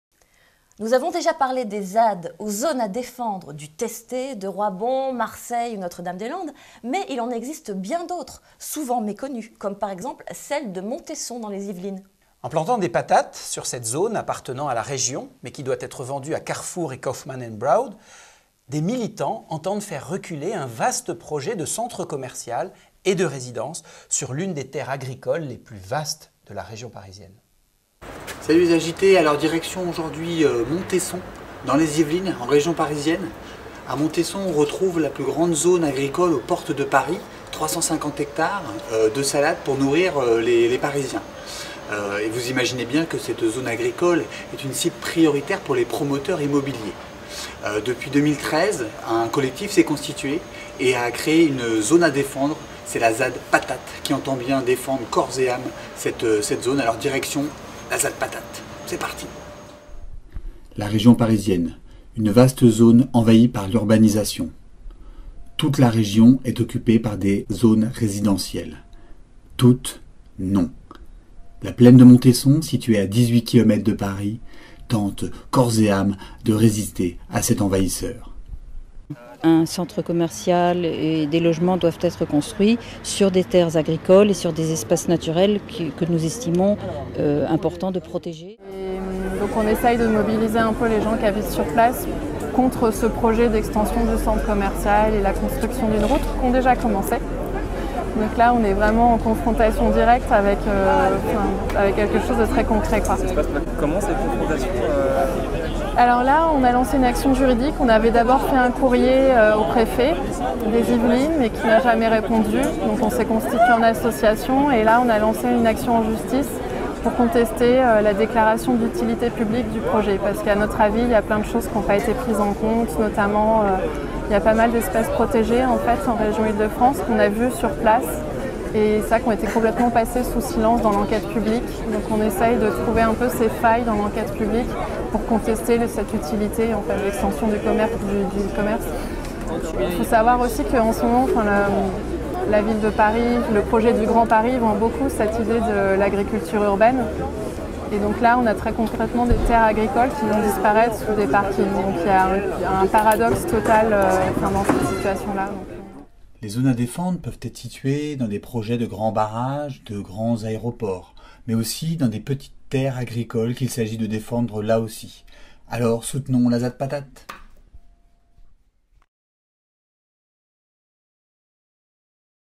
Zad patate à Montesson - Reportage AlterJT 20/03/15 - 24
A Montesson dans les Yvelines, un collectif s'est constitué pour lutter contre l'urbanisation de la Plaine. En plantant des patates sur cette zone à défendre (zad), ils entendent bien faire reculer un vaste projet de centre commercial et de résidence sur l'une des terres agricoles les plus vastes de la région parisienne.